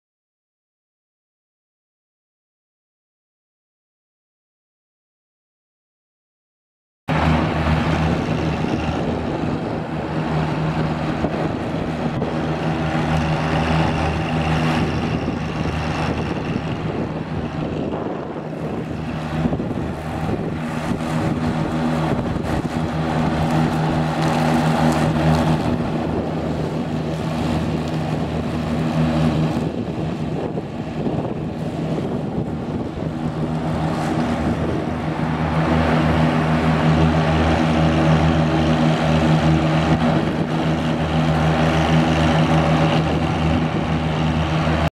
John Deere 856 lawn mower for sale | sold at auction June 3, 2015
John Deere 856 lawn mower
1,672 hours on meter
72" cut
Yanmar three cylinder diesel engine
19.9 kW
Hydrostatic transmission
Four wheel drive
Work lights
23x10.50-12 front tires
18x8.50-10 rear tires
Manuals
Serial TC1445D012531

It was located in Richmond, KS, and we sold it to the highest bidder for $6,270.